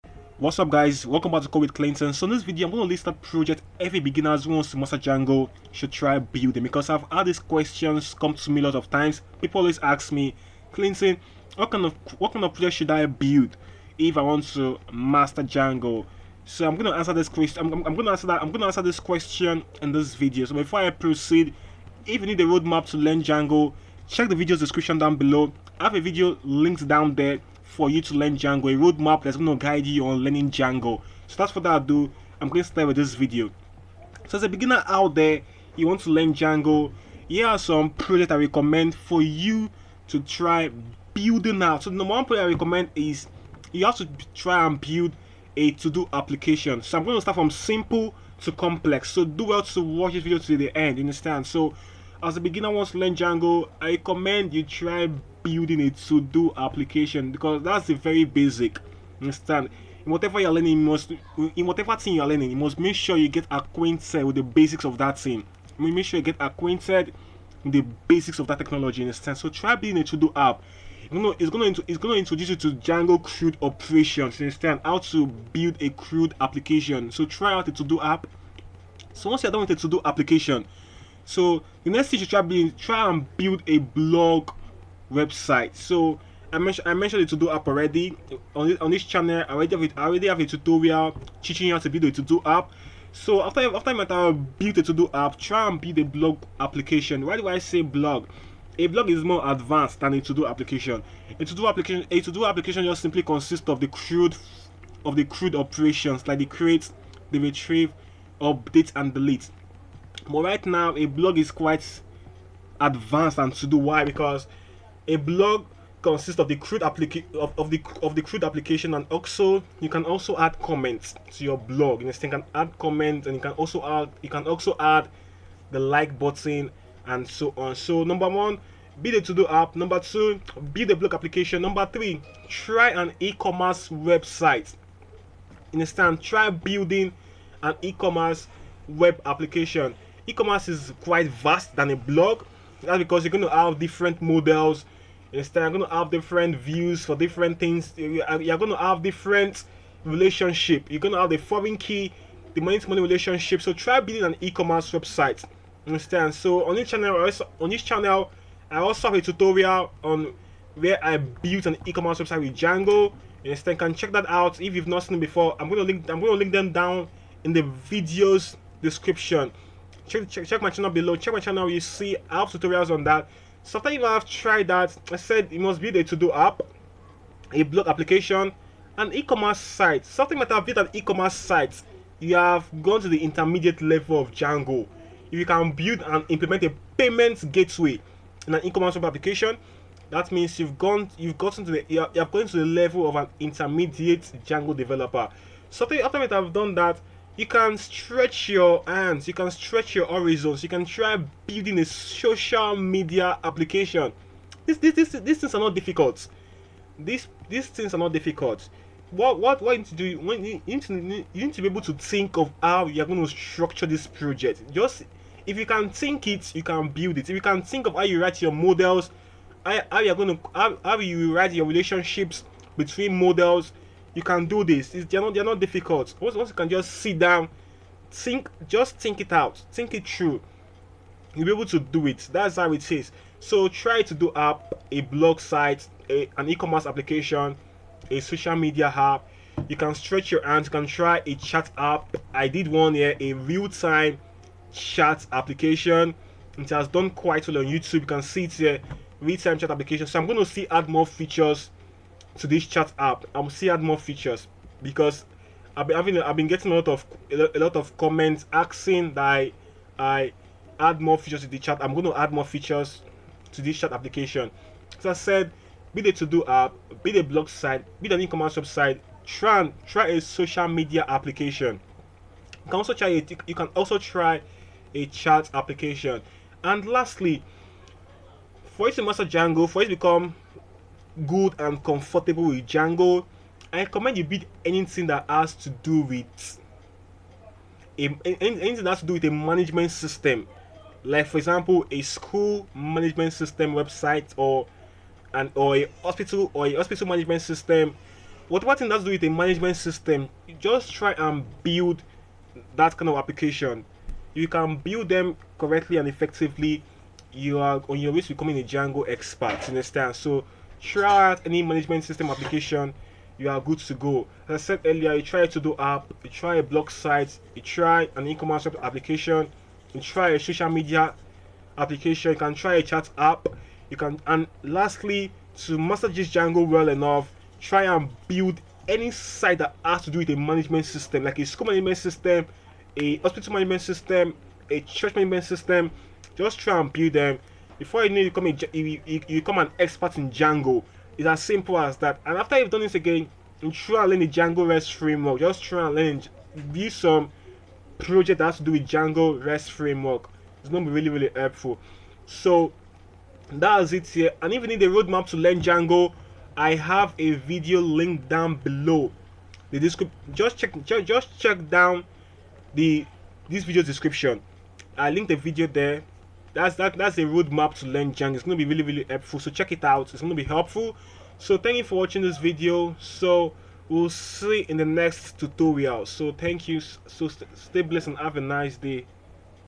Django Projects Ideas for Beginners to Advanced.
Hi, are you a beginner learning Django and don't know the suitable projects to build? Worry no more, in this video I will list out the projects you need to build that will help you grow from a beginner to an expert Django developer.